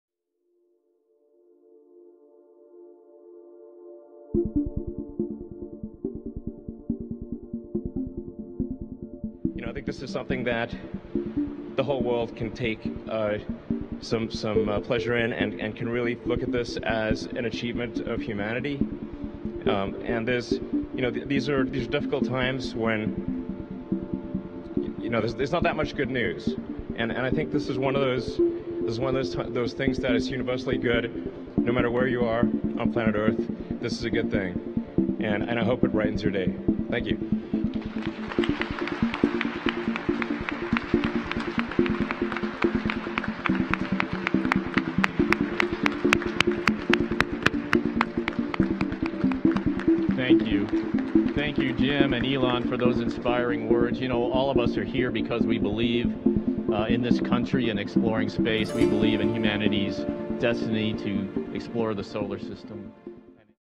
SpaceX Crew Dragon Demo 2 Splash-down Footage in 4k, Elon speech edit
NASA released new footage of SpaceX Crew Dragon Endeavor splashing down in the Gulf of Mexico on August 2nd. This quick edit mixes inspirational words from Elon Musk who, minutes after splashdown, flew  to Johnson Space Center in Houston to meet with Astronauts Bob Behnken and Doug Hurley in person.